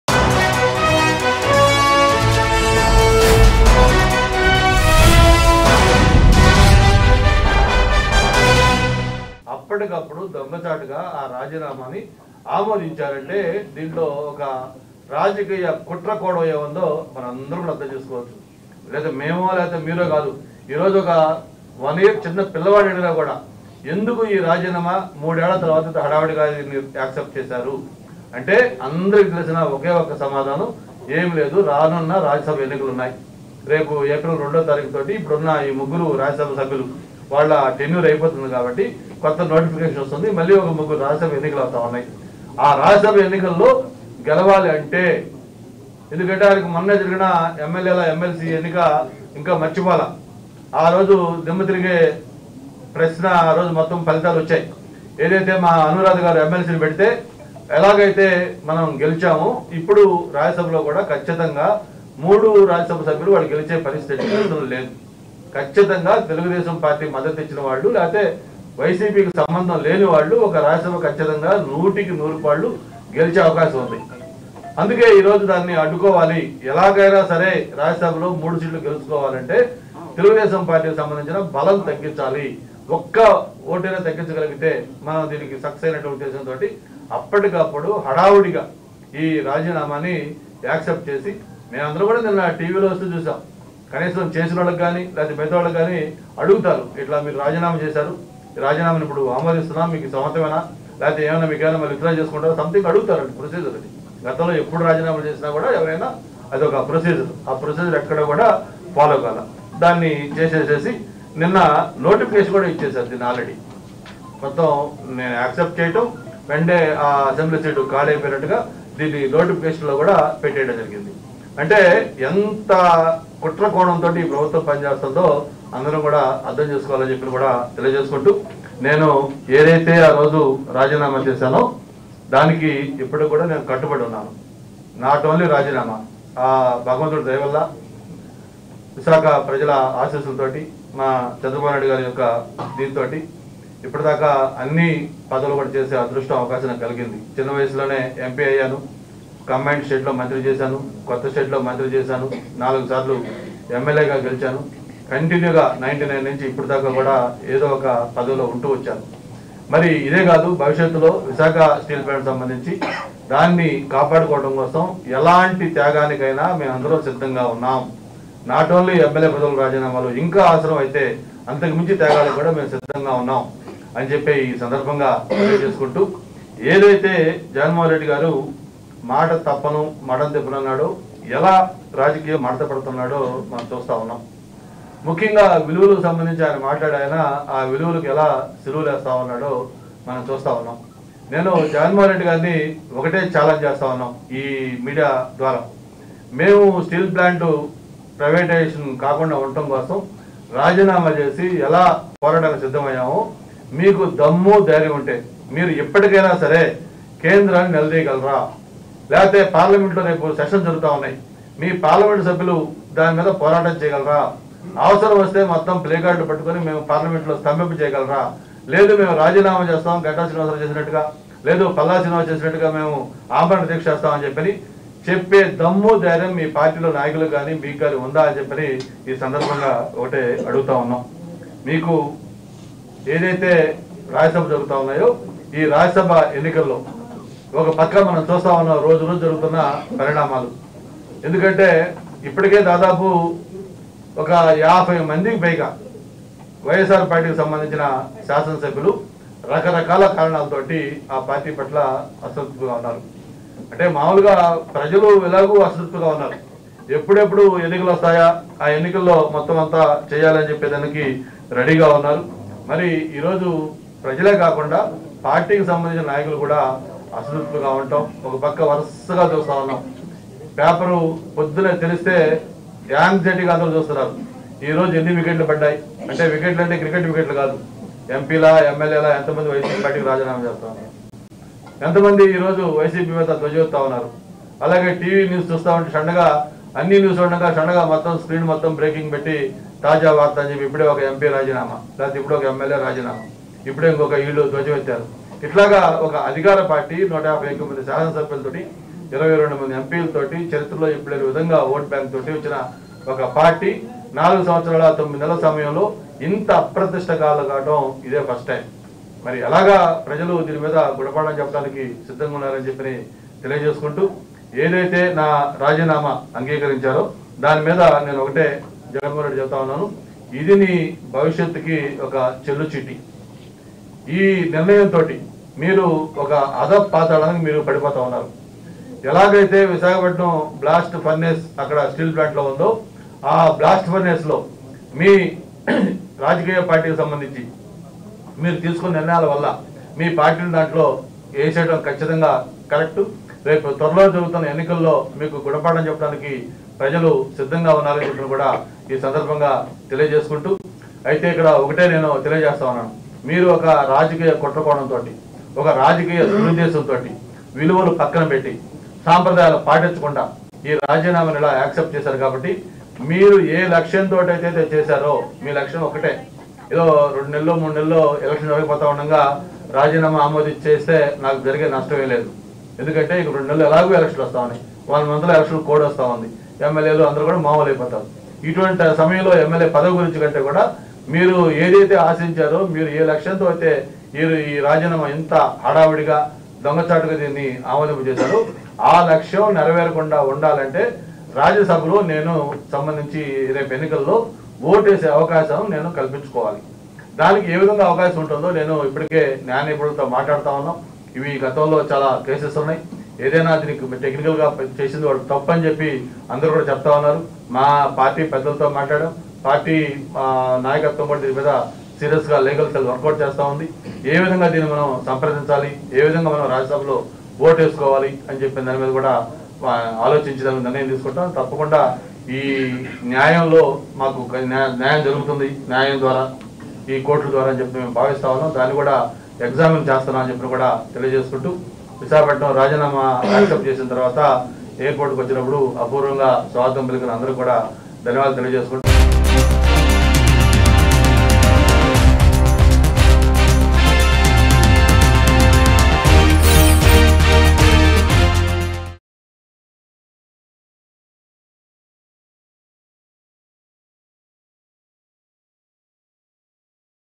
TDP Leader Ganta Srinivasa Rao Press Meet On Governor Accepting Resignation | AP News | Mango News
TDP Leader Ganta Srinivasa Rao Press Meet On Governor Accepting Resignation.